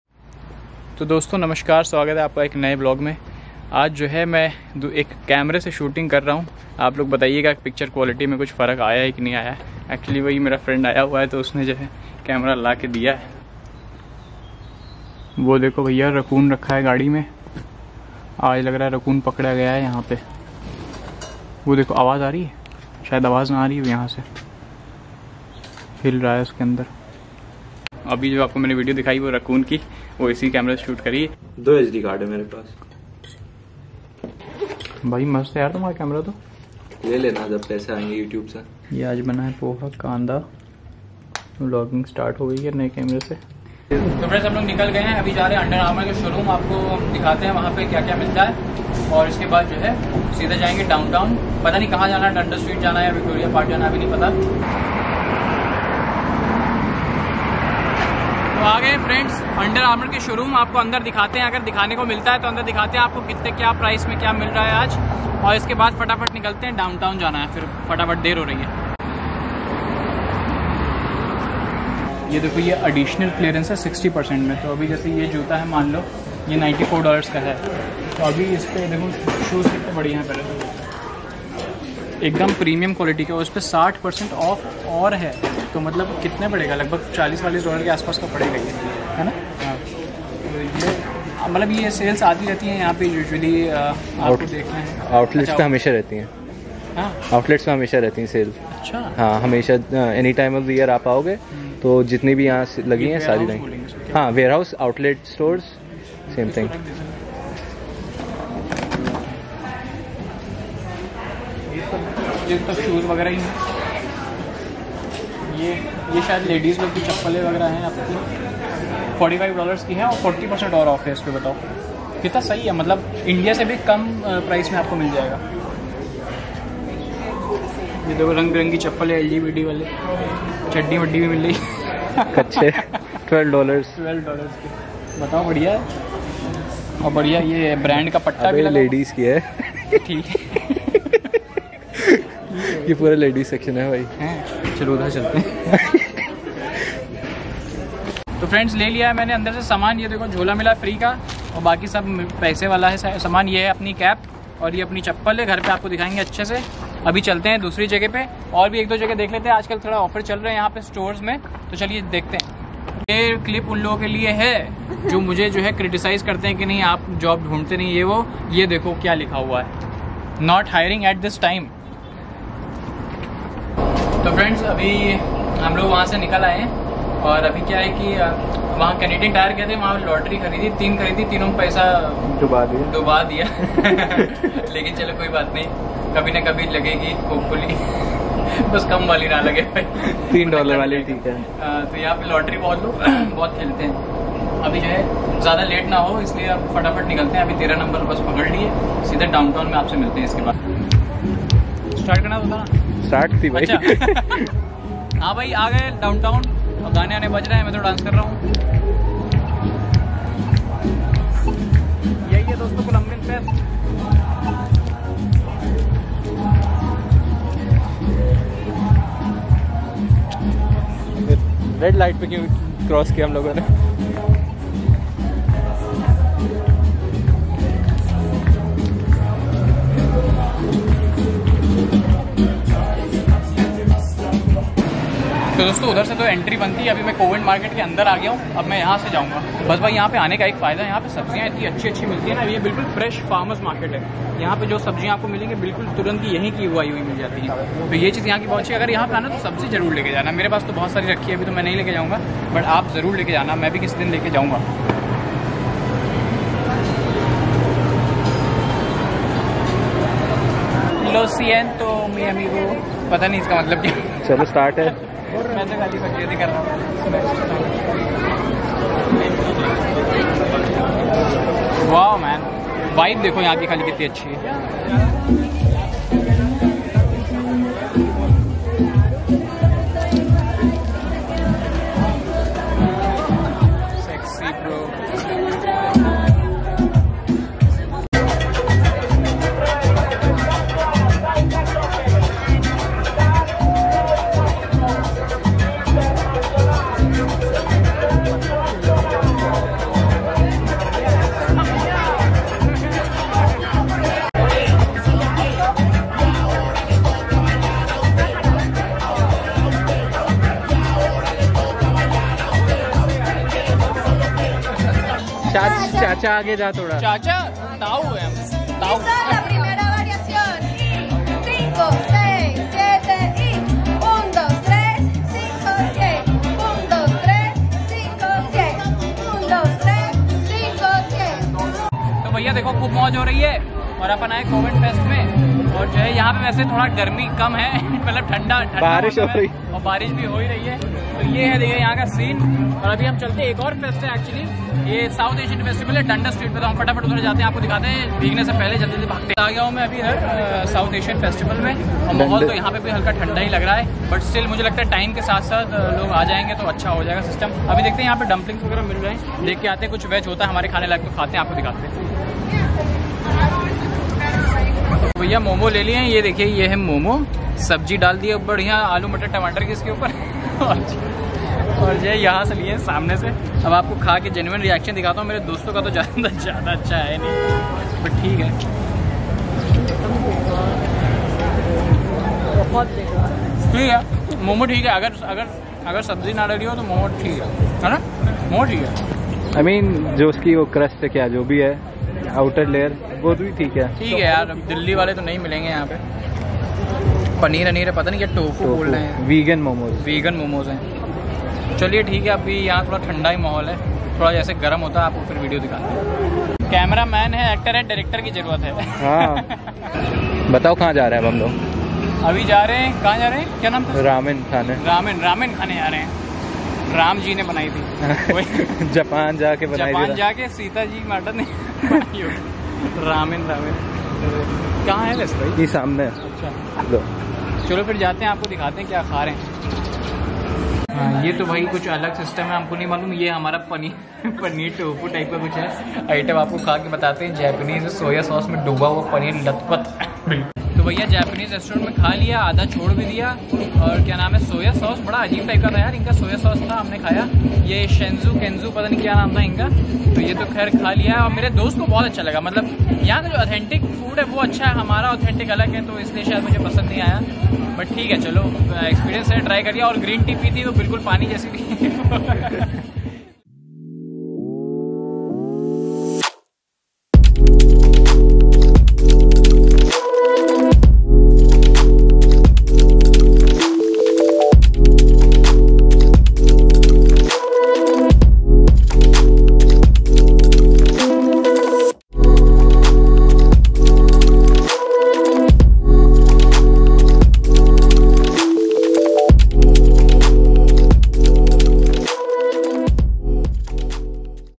VLOG 26 | INDIAN AND COLOMBIAN FEST | UNDER ARMOUR SALE | LONDON DOWNTOWN
Hey people, I am an international student in London, Ontario in Fanshawe college. I make regular vlogs and informative videos to help and educate people aspiring to come to London.